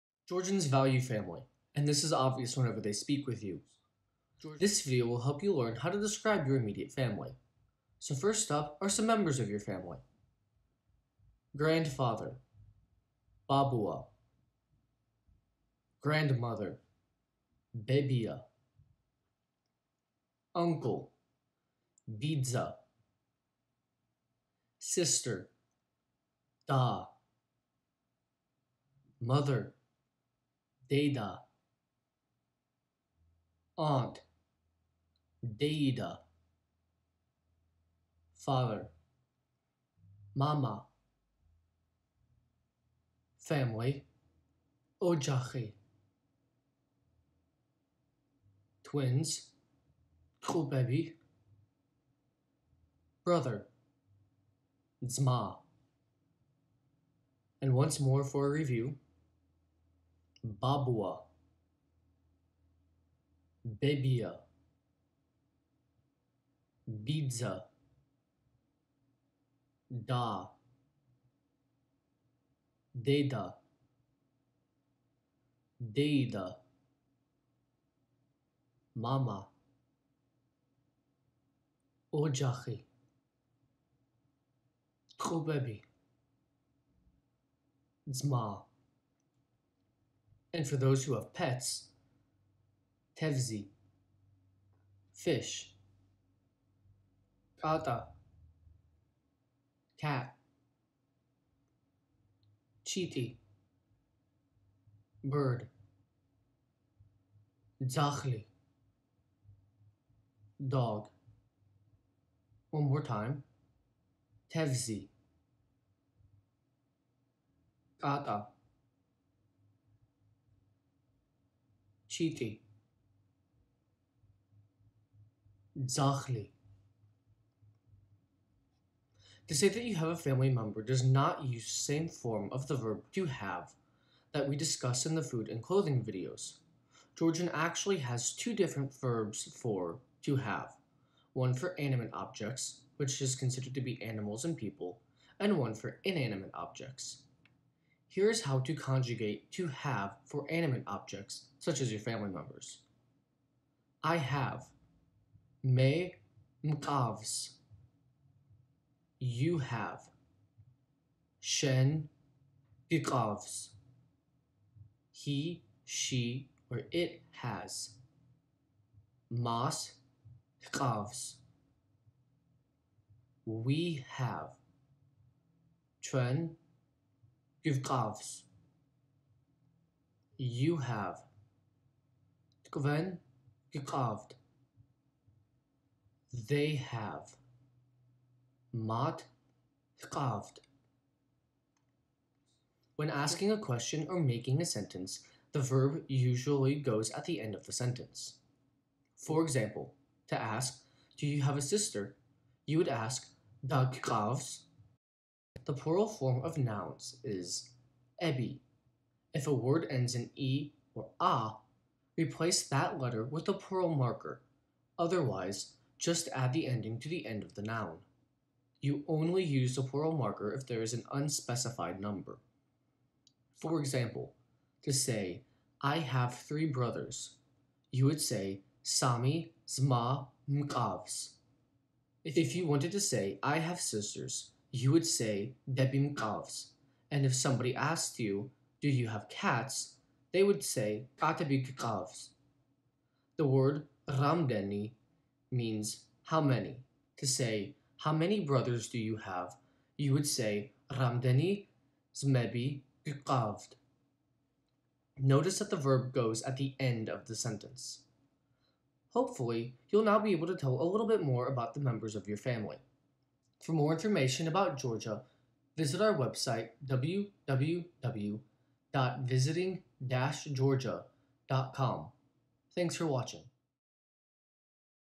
Family
This video goes over how to talk about your family members in Georgian, how to say "to have," the plural ending, and reviews the word რამდენი "how many."